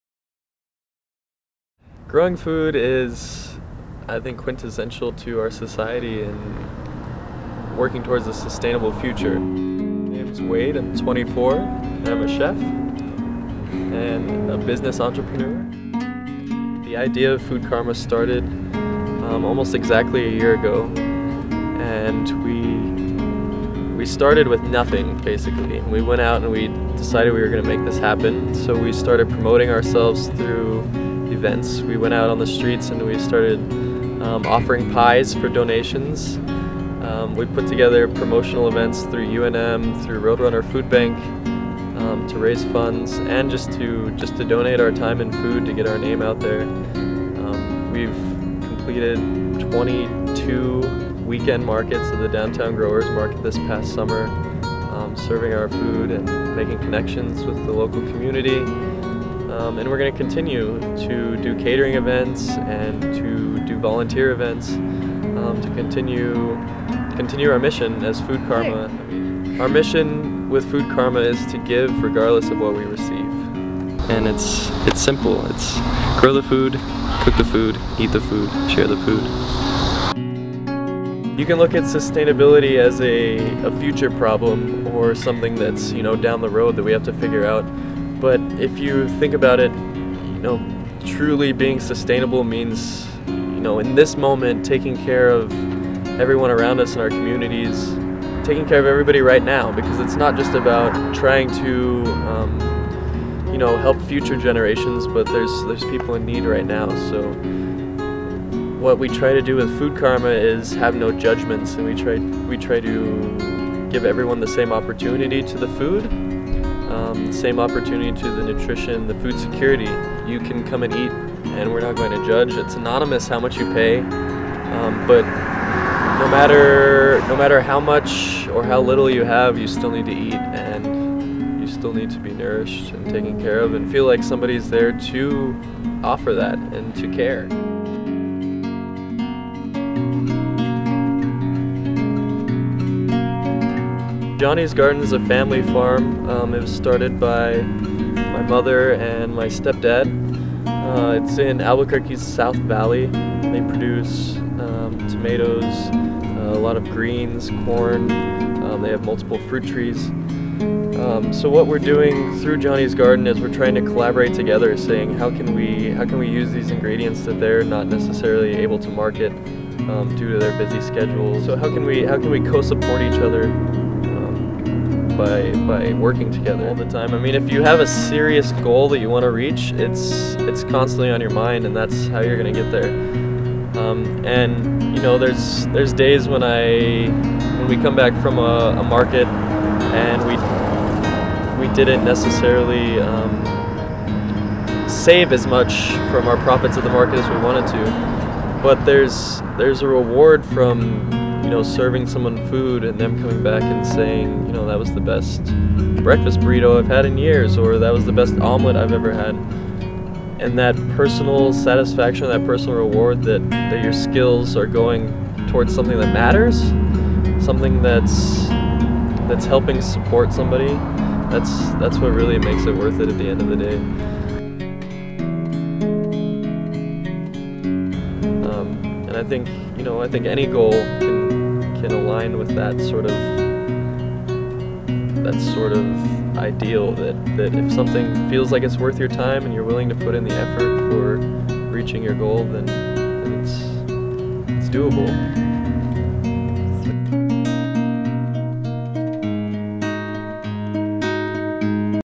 Food Karma for NMFMA
This is a video provided by JAK Media to the New Mexico Farmer's Market Association. Our goal was to represent and honor the fantastic work this young entrepreneur has done for our community.